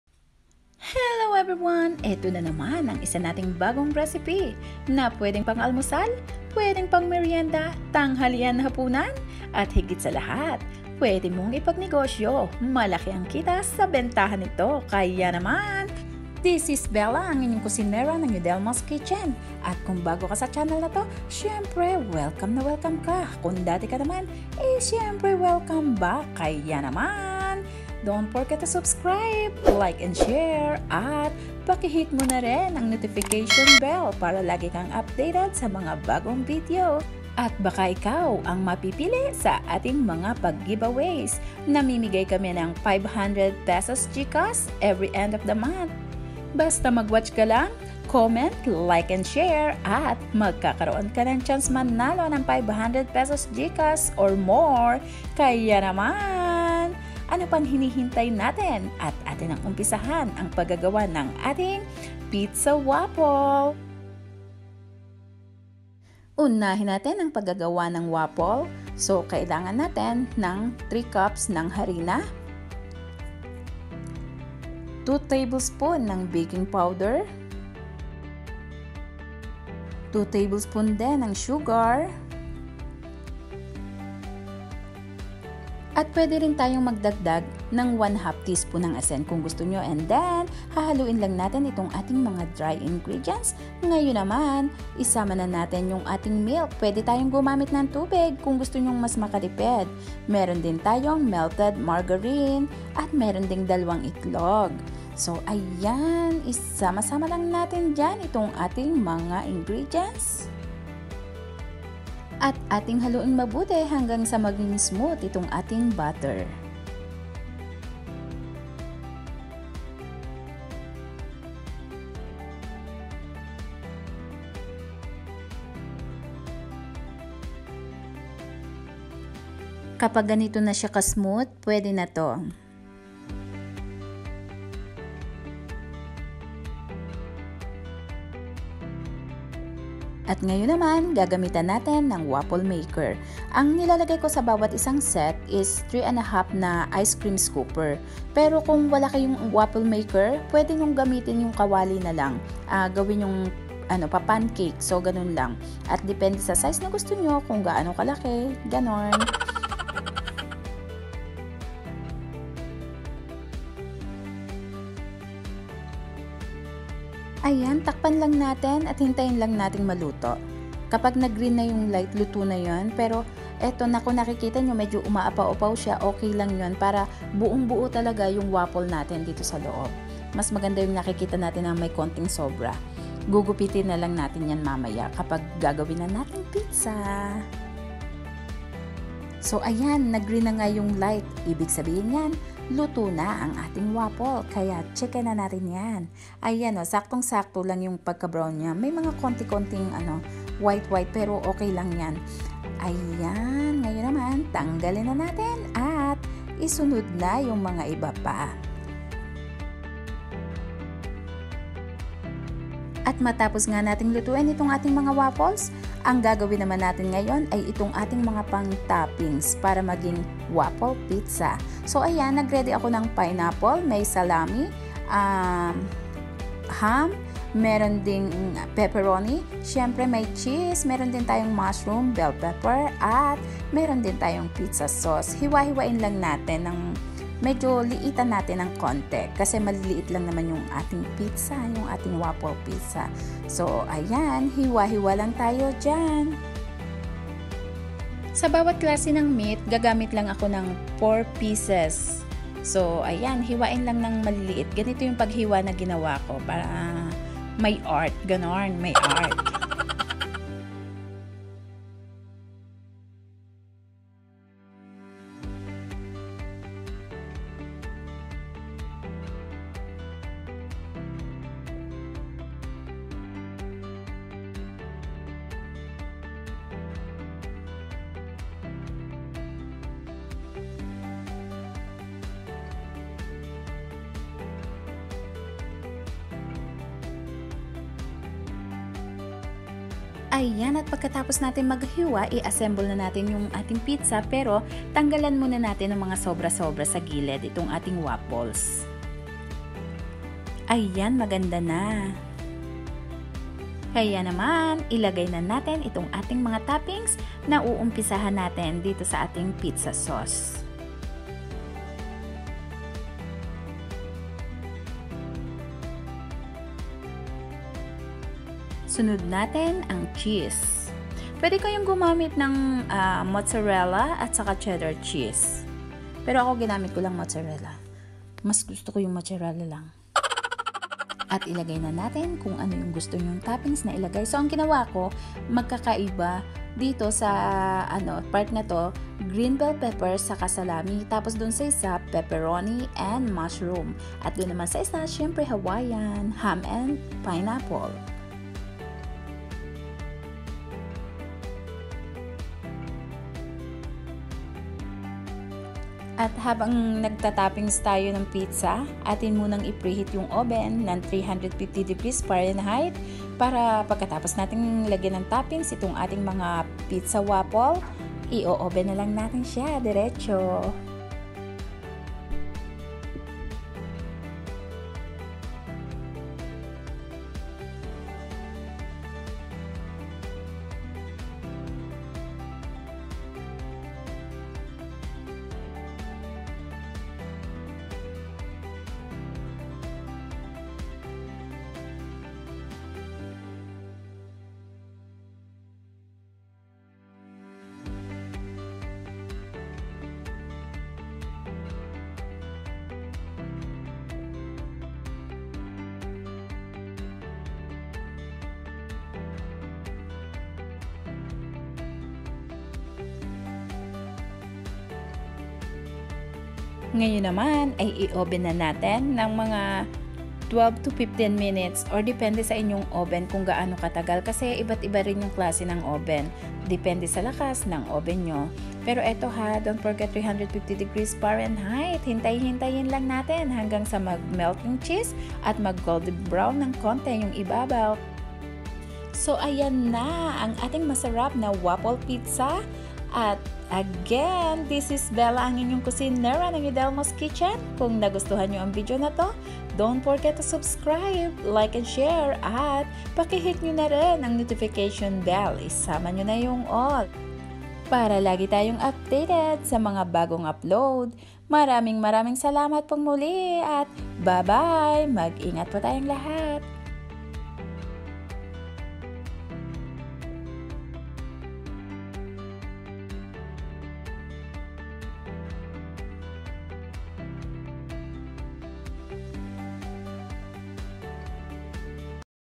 Pizza Waffle! Itry Natin ang KOREAN STREET FOOD | Yudelmo’s Kitchen
Ingredients:

WAFFLE:
3 cups all purpose flour
2 tbsp baking powder
2 tbsp sugar
1/2 tsp salt
2 1/2 cup milk, you can also use 50/50 milk and water or just water instead of milk
1/2 cup melted margarine or butter
2 eggs

PIZZA:
you can use any toppings of your choice

I use
regular pizza sauce
salami
ham
pepperoni
pineapple
green bell pepper
mushroom
mozzarella cheese

THANKS